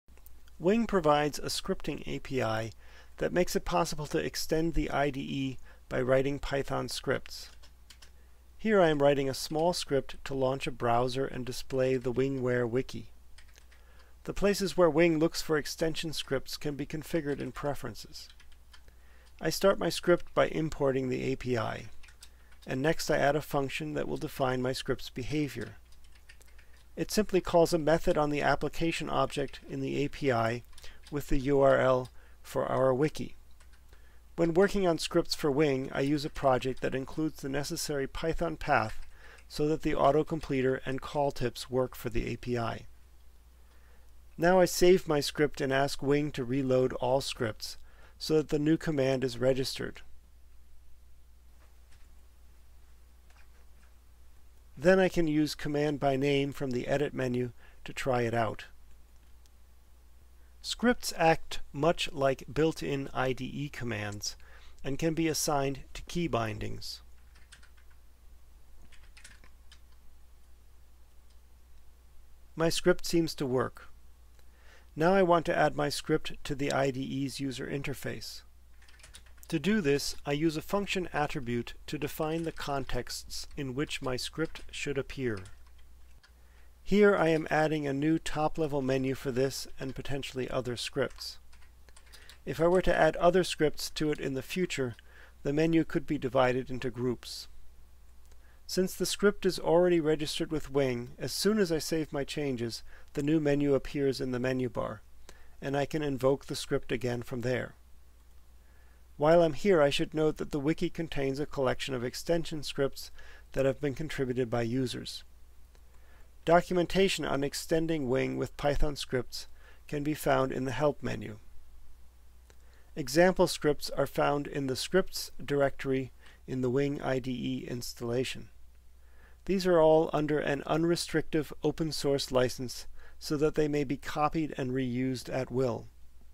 Extending Wing IDE with Python Scripts
Briefly shows extending Wing IDE with a Python script.